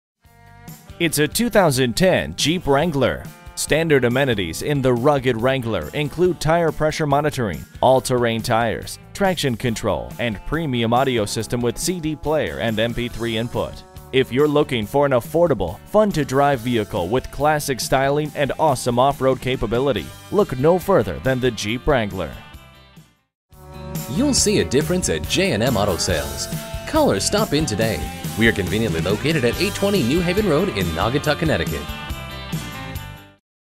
2010 Jeep Wrangler 4wd Naugatuck CT Hartford, CT #108434 - SOLD
Year: 2010
Make: Jeep
Model: Wrangler 4wd
Trim: 2d Convertible Sport
Engine: -1 Cylinders
Color: White
Mileage: 60350
Stock #: 108434
VIN: 1J4AA2D19AL108434
An Impressive, Four Wheel Drive Wrangler Sport with an Accident-Free History Report and an Included Warranty! It's Nicely Equipped with a Hard Top, Grey Interior, Alloy Wheels, Air Conditioning, a CD Player, Power Windows, Power Door Locks, Power Mirrors, an Adjustable Steering Column, Cruise Control, a 3.8L V6 Engine, an Automatic Transmission and More! A Quality Choice at J&M!.

Address:
820 New Haven Rd.